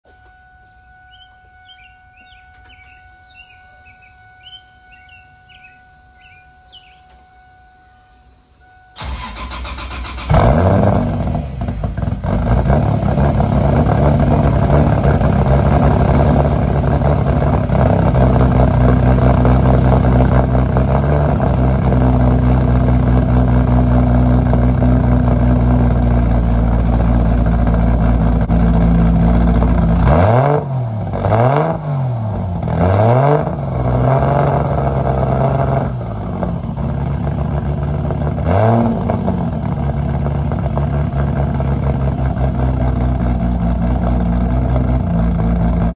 1jz-gte Supra 4” exhaust cold start
Cold start, idle and rev of my 1990 1jz swapped Supra.

Engine is a fresh rebuild, 9.5:1 CP pistons, balanced engine, 264 HKS cams, AEM EMS V1, Sard 840cc side feed inj, GT4088 turbo, full 4” turboback exhaust.